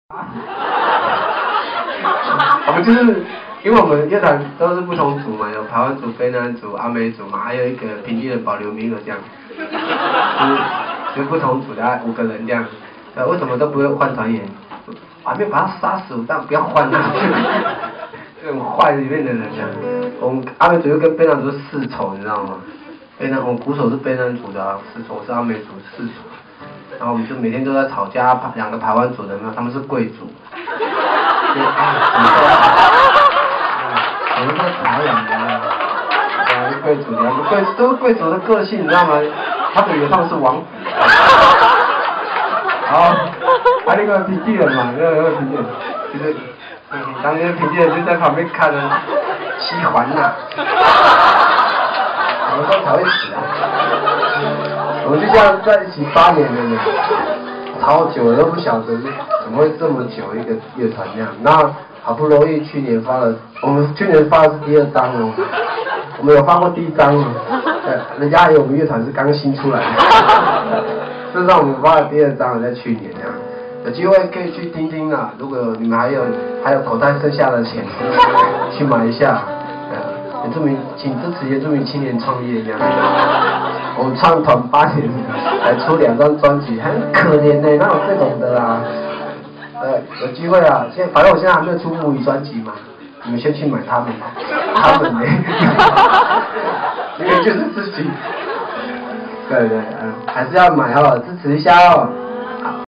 99.01.21 法國河岸小酒吧-suming請你記得我之"圖騰樂團在一起八年"
阿美跟卑南是世仇,兩個排灣是貴族~他們以為他們是王子
如果還有口袋剩下的錢,去買一下!!
說到圖騰一直說他們~明明就是自己 ^ ^